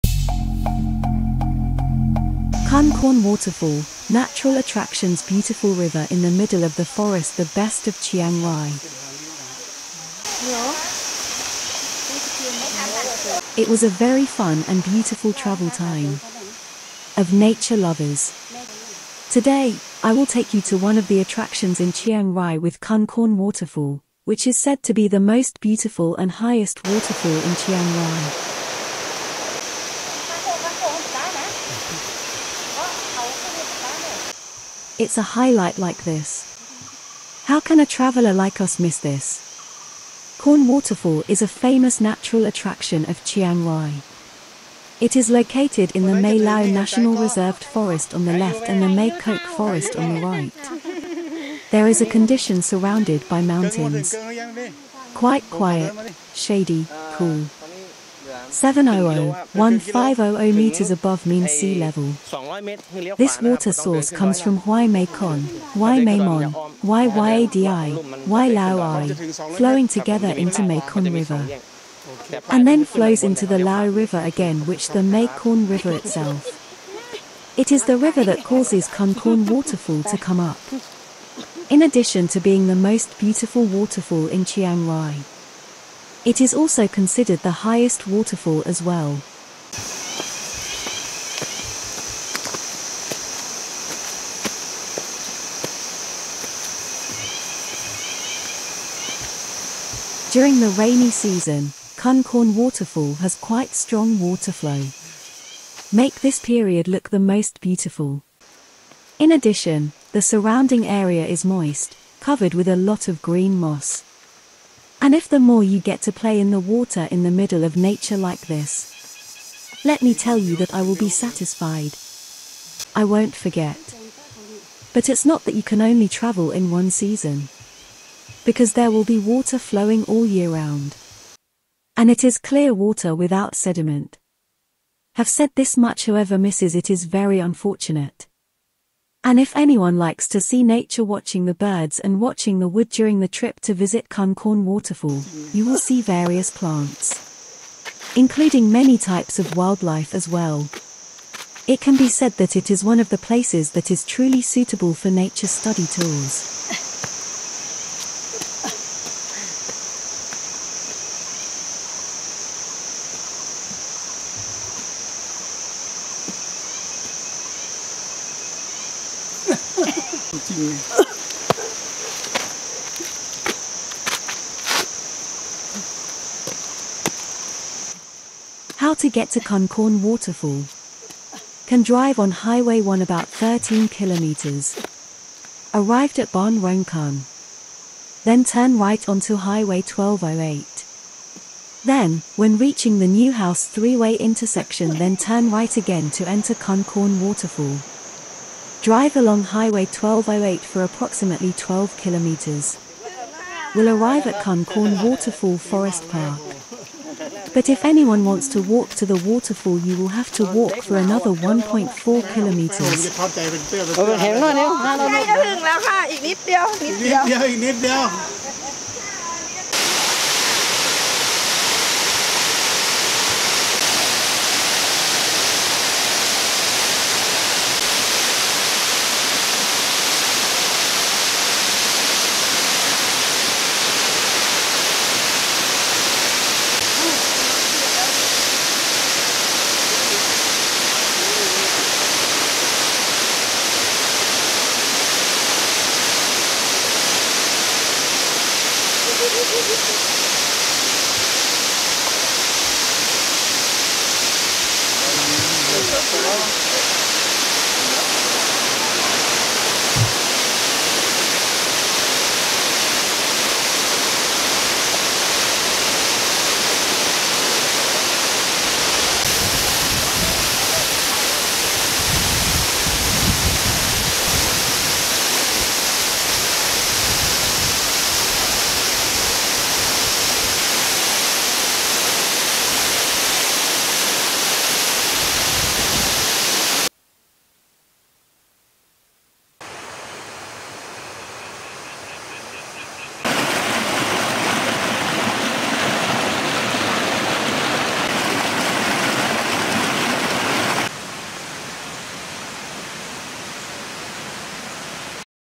Khun Korn Waterfall, natural attractions Beautiful river in Chiang Rai
Khun Korn Waterfall, natural attractions Beautiful river in the middle of the forest The best of Chiang Rai

It was a very fun and beautiful travel time. of nature lovers
Today, I will take you to one of the Attractions in Chiang Rai with Khun Korn Waterfall, which is said to be the most beautiful and highest waterfall in Chiang Rai.
It's a highlight like this. How can a traveler like us miss this?

Khun Korn Waterfall Khun Korn Waterfall is a famous natural attraction of Chiang Rai
It is located in the Mae Lao National Reserved Forest on the left and the Mae Kok Forest on the right.
There is a condition surrounded by mountains. Quite quiet, shady, cool.
700-1500 meters above mean sea level

This water source comes from Huai Mae Kon, Huai Mae Mon, Huai Ya Di, Huai Lao Ai, flowing together into Mae Kon River.
and then flows into the Lao River again which the Mae Korn River itself
It is the river that causes Khun Korn Waterfall to come up.
In addition to being the most beautiful waterfall in Chiang Rai. It is also considered the highest waterfall as well.

During the rainy season, Khun Korn Waterfall has quite strong water flow.
  Make this period look the most beautiful.
In addition, the surrounding area is moist, covered with a lot of green moss.
And if the more you get to play in the water in the middle of nature like this
Let me tell you that I will be satisfied. I won't forget. But it's not that you can only travel in one season. because there will be water flowing all year round
And it is clear water without sediment.
have said this much Whoever misses it is very unfortunate.

And if anyone likes to see nature Watching the birds and watching the wood During the trip to visit Khun Korn Waterfall, you will see various plants. Including many types of wildlife as well
  It can be said that it is one of the places that is truly suitable for nature study tours.

How to get to Khun Korn Waterfall
can drive on Highway 1 about 13 kilometers
Arrived at Rong Khun House. Then turn right onto Highway 1208.
Then, when reaching the new house three-way intersection then turn right again to enter Khun Korn Waterfall
Drive along Highway 1208 for approximately 12 kilometers.
will arrive at Khun Korn Waterfall Forest Park
But if anyone wants to walk to the waterfall You will have to walk for another 1.4 kilometers..